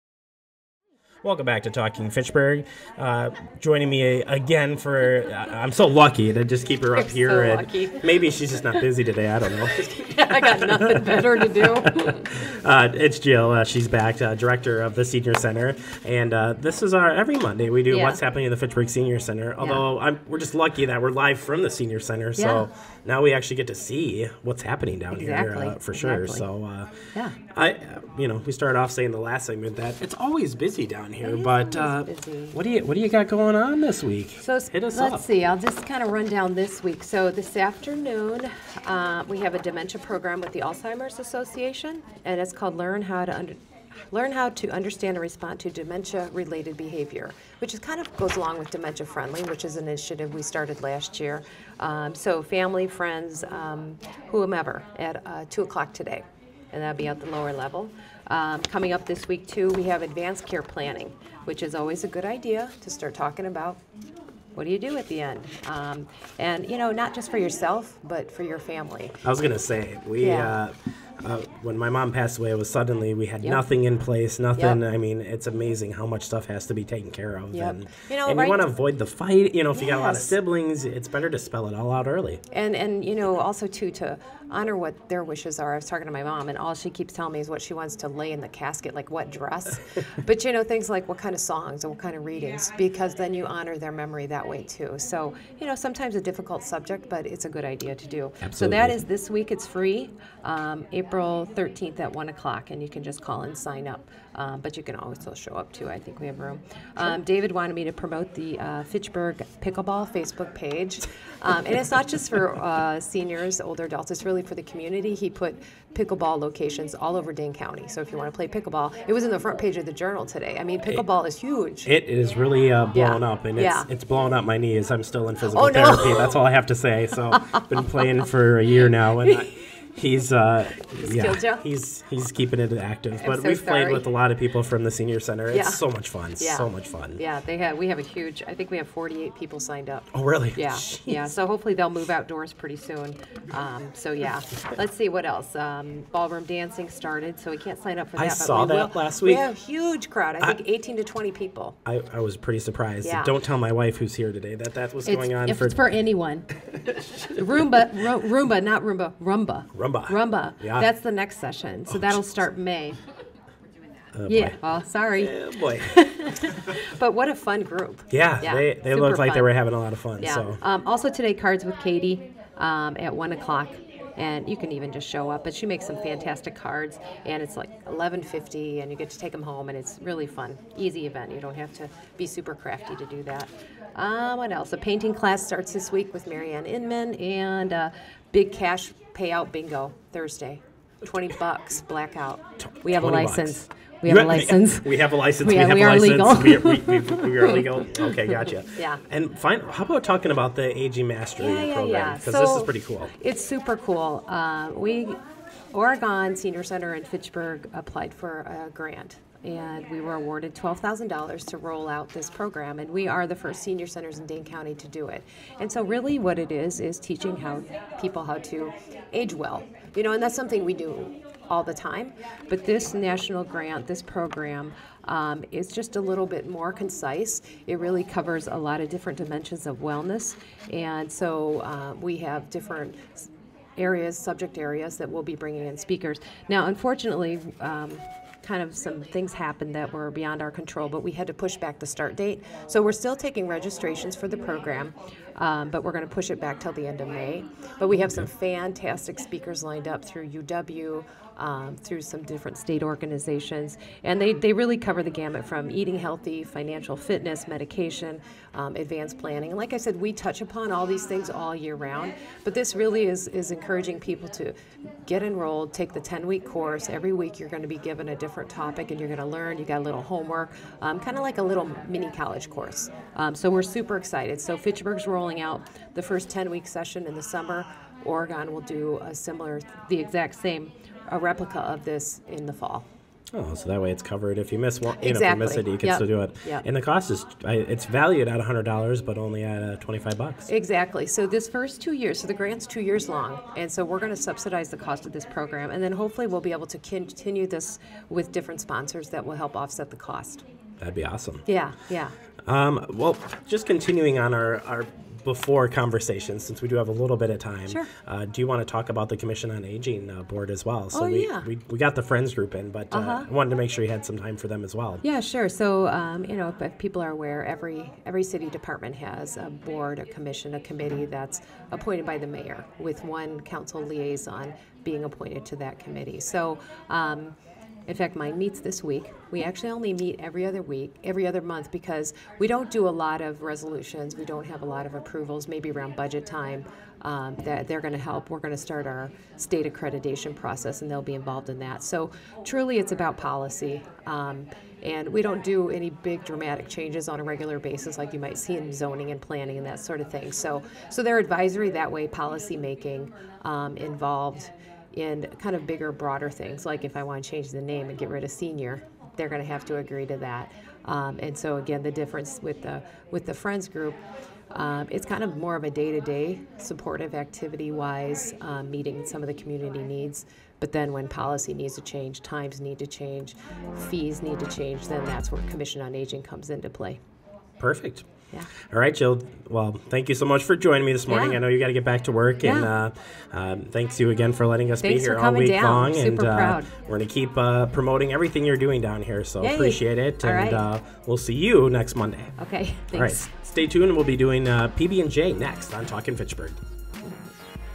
Talking Fitchburg: What's Happening at the FSC
Get your weekly update from the Fitchburg Senior Center.